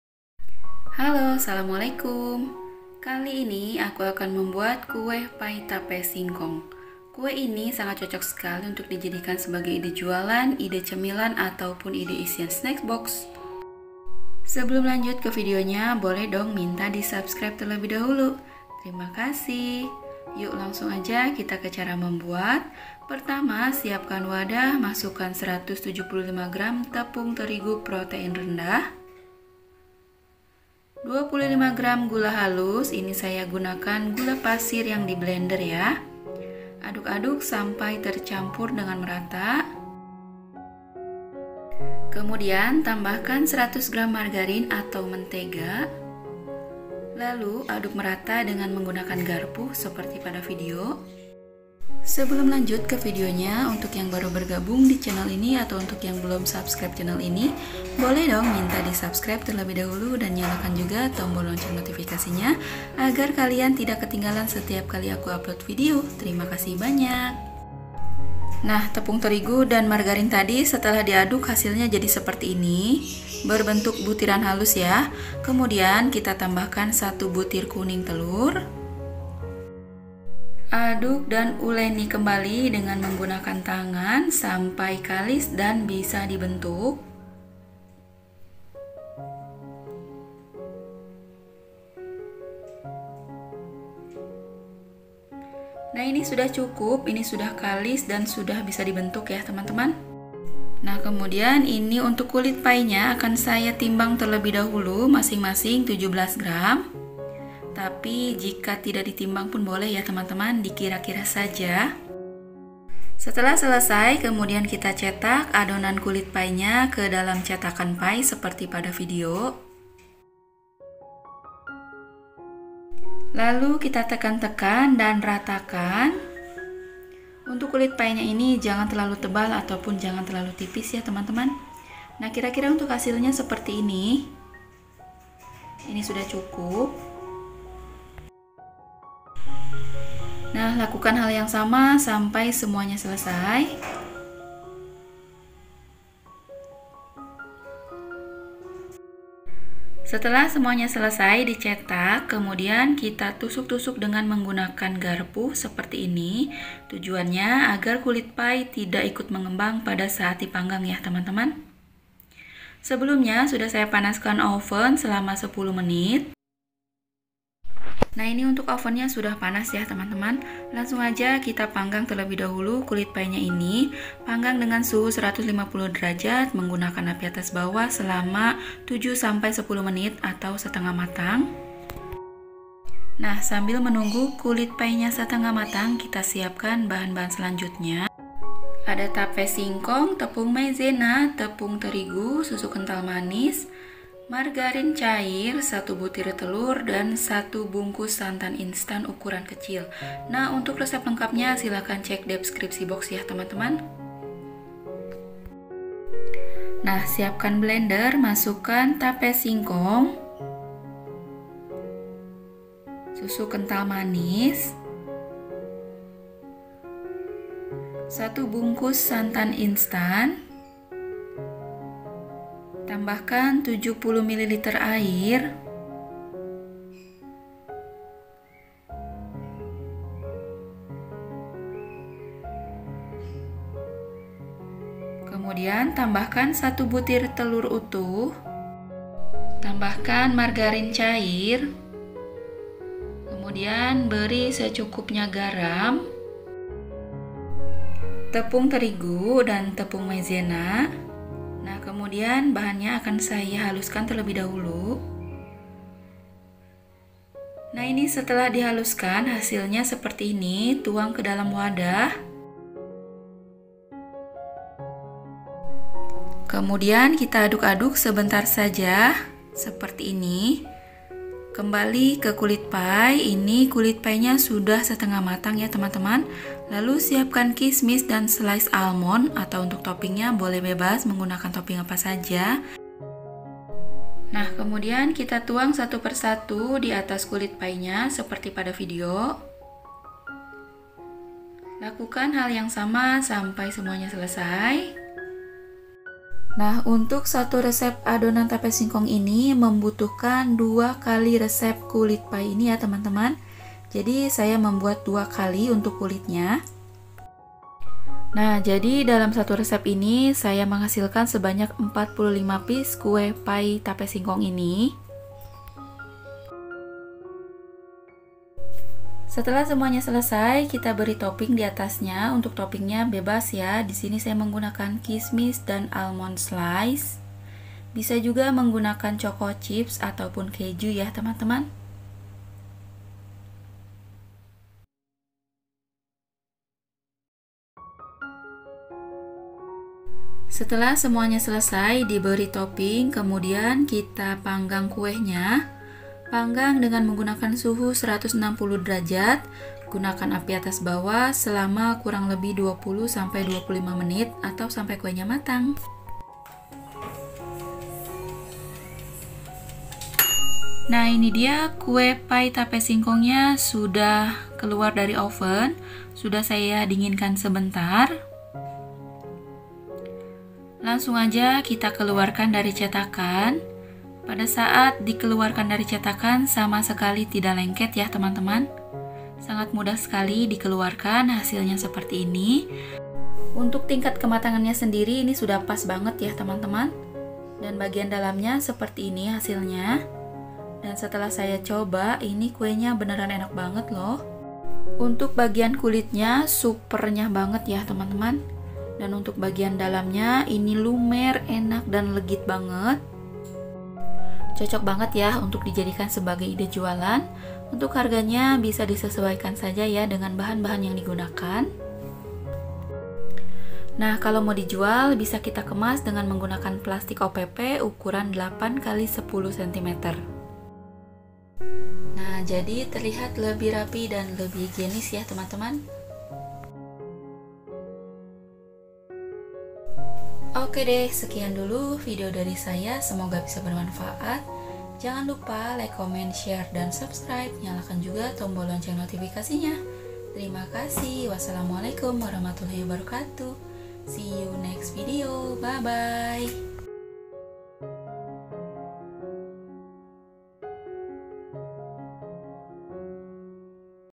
AKU KASIH 1 BUAT TESTER,GAK NYANGKA LANGSUNG DI BORONG || SEENAK ITU❗
Assalamualaikum wr.wb

Kali ini aku mau buat kue pie tape singkong,,olahan tape singkong ini ternyata rasanya enak bangett lho .. Kulitnya renyah banget dan bagian dalamnya lumerrr bangett..Perpaduan yang sangat pas ya ..

KUE PIE TAPE SINGKONG
Bahan kulit :
175gr tepung terigu pro rendah (me:kunci biru)
25gr gula halus (sebaiknya gunakan gula pasir yg di haluskan)
100gr margarin / butter
1 butir kuning telur

Bahan tape singkong:
200gr tape singkong (buang sumbunya)
25gr gula halus
50gr SKM putih
50gr margarin / mentega (lelehkan)
1butir telur utuh
1bgks santan instan
70ml air.
Garam secukupnya
20gr Tepung maizena
20gr Tepung terigu

Topping : kismis & almond slice

Ket :
Untuk 1 resep adonan tape membutuhkan 2x resep adonan kulit pie (bahan kulit pie di atas dikali 2 ya).